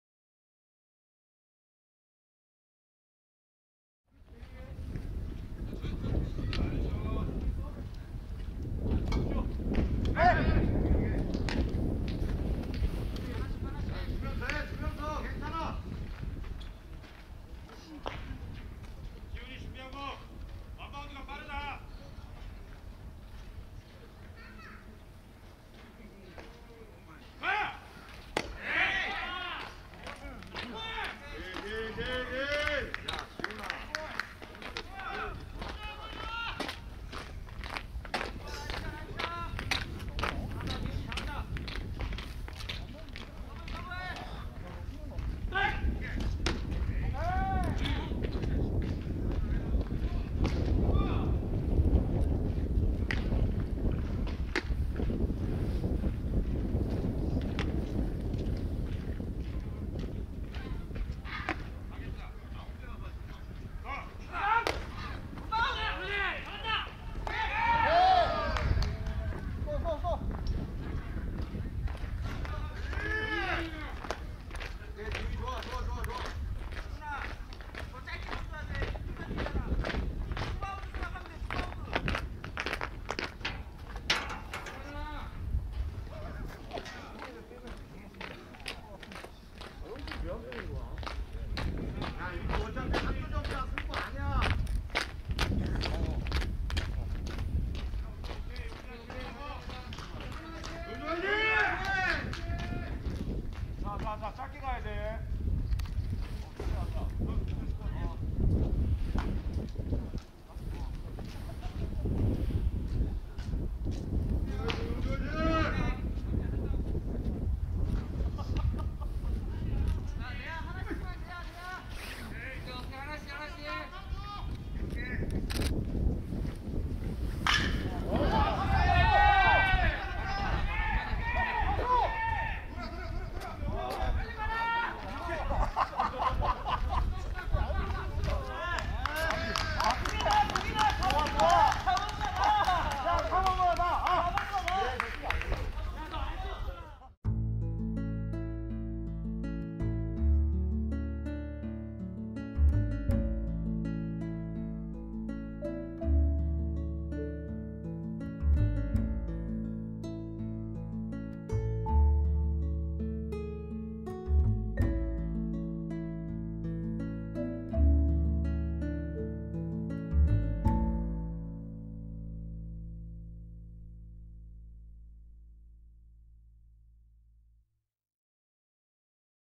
아스카론 야구단 임우제 타격영상 - 사회인야구 - 제8회 화성시장배 전국 사회인야구대회 -
제8회 화성시장배 전국 사회인야구대회 선출부 결승전
HS밴더스와의 경기에 아스카론 야구단 3번타자로 출전한
임우제 선수의 타격 영상입니다.
임우제 선수는 이 경기에서 4타수 3안타 2타점 3득점으로 활약 했습니다.